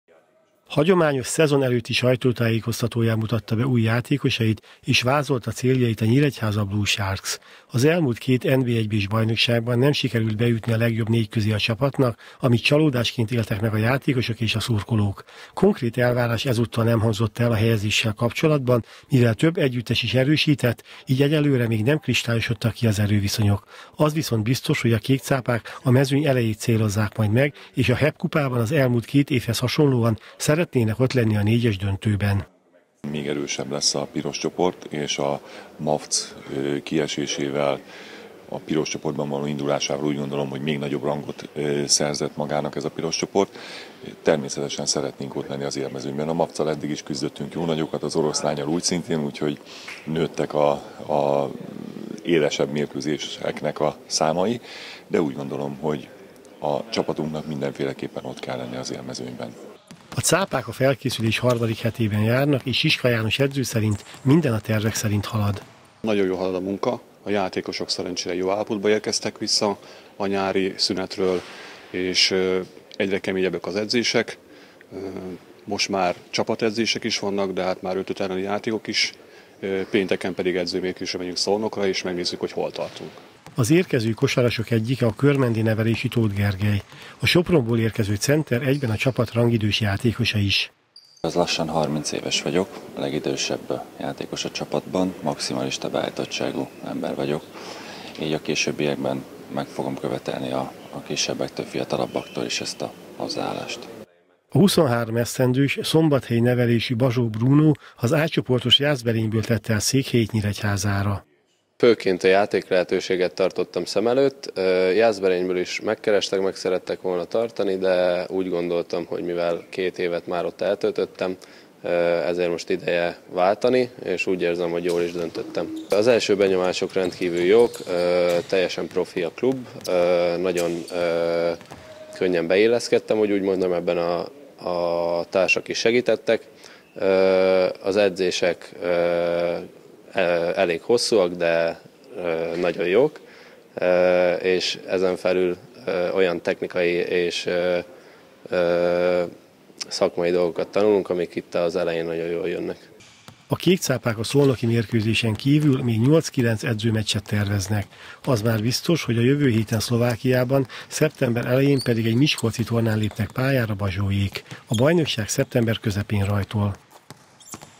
Az élmezőny a cél - Kölcsey Tv
Az élmezőnyben szeretne végezni a következő szezonban az NB I/B Piros csoportjában a Nyíregyháza Blue Sharks. A Kék Cápák szerdán sajtótájékoztatón mutatták be két új játékosukat, a Sopronból érkező Tóth Gergelyt és a legutóbb Jászberényben szereplő Bazsó Brúnót. Siska János együttese pénteken Szolnokon játsza első felkészülési meccsét, a bajnokság szeptember közepén rajtol.